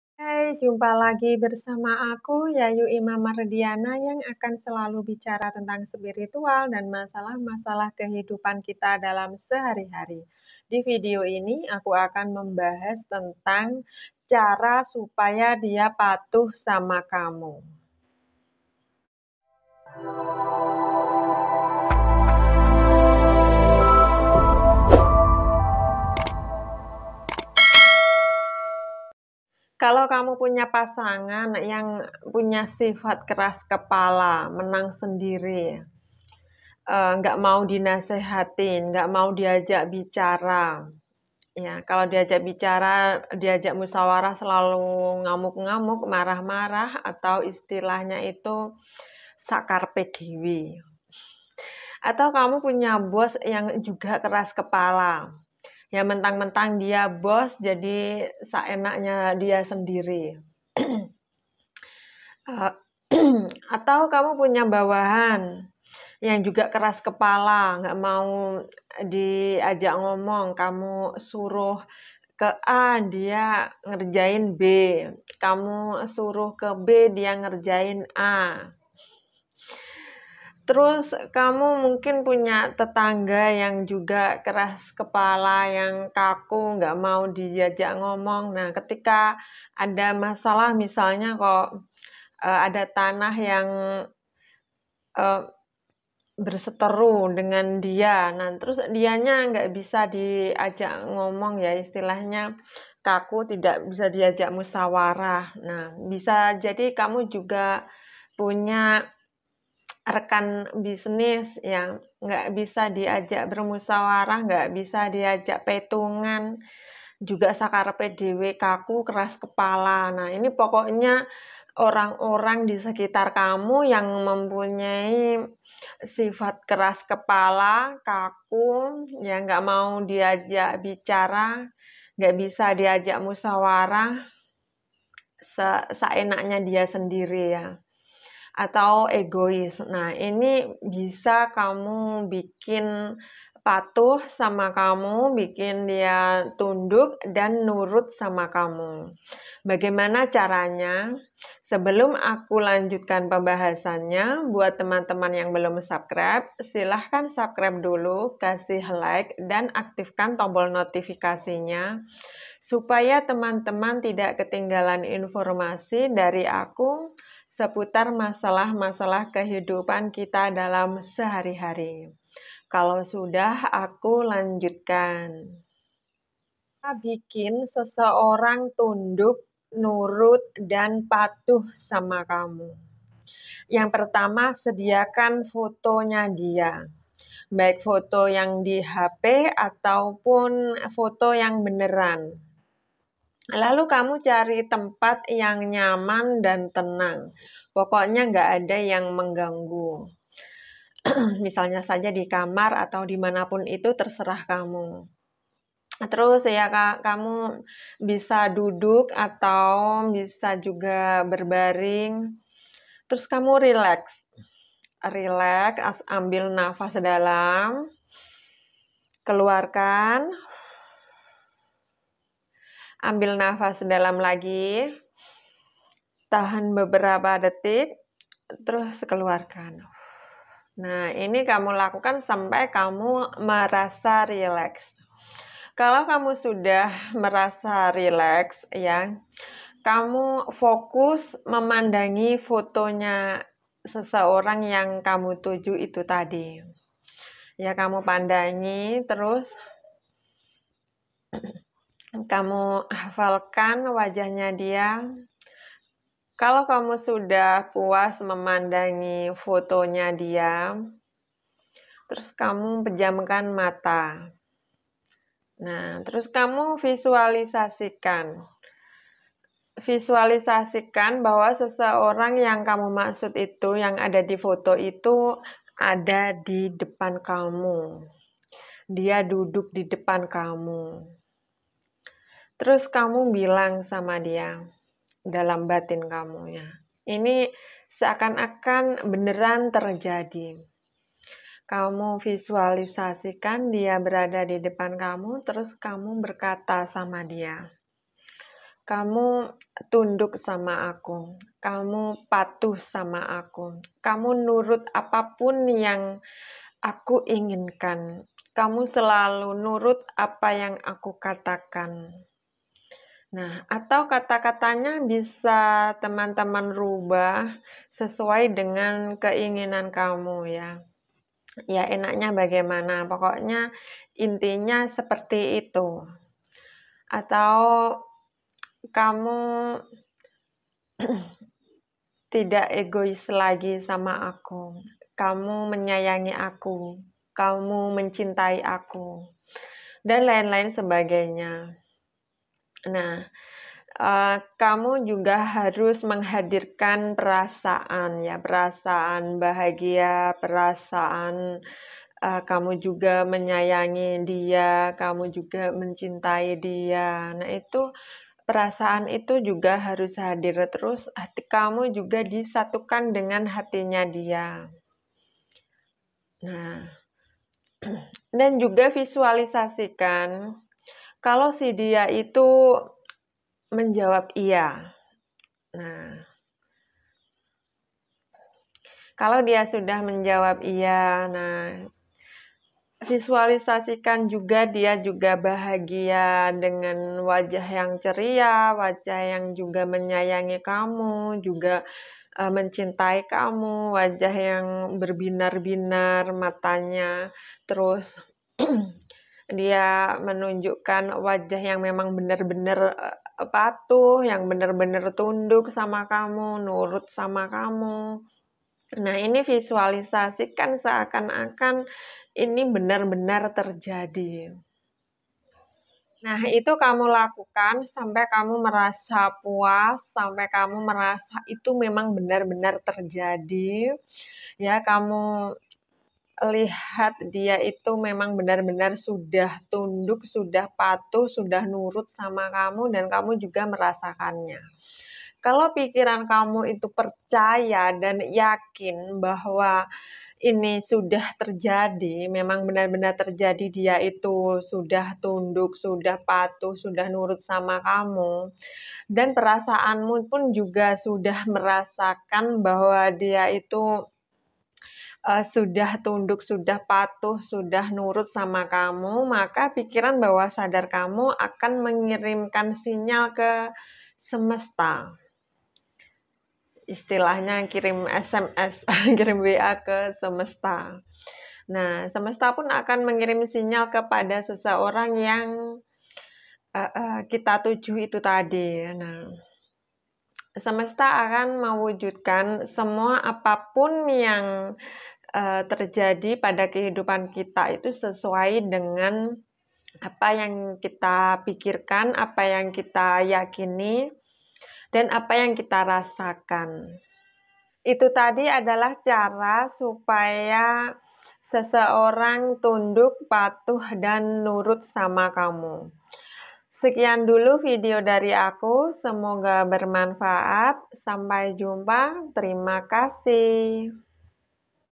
supaya dia patuh sama kamu|bikin dia tunduk dan nurut sama kamu
Hanya teman teman yang pusing menghadapi pasangan yang keras kepala, atau punya bos yang seenaknya sendiri, atau punya bawahan yang sulit diajak ngomong, atau punya tetangga yang keras kepala tk bisa di ajak musyawarah atau punya partner bisnis yang juga keras kepala, bikin mereka orang orang yang keras kepala jadi patuh sama kamu, jadi tunduk dan nurut sama kamu. Cara nya nonton video ini, karena video ini menjelaskan cara supaya dia patuh sama kamu, bikin dia tunduk dan nurut sama kamu.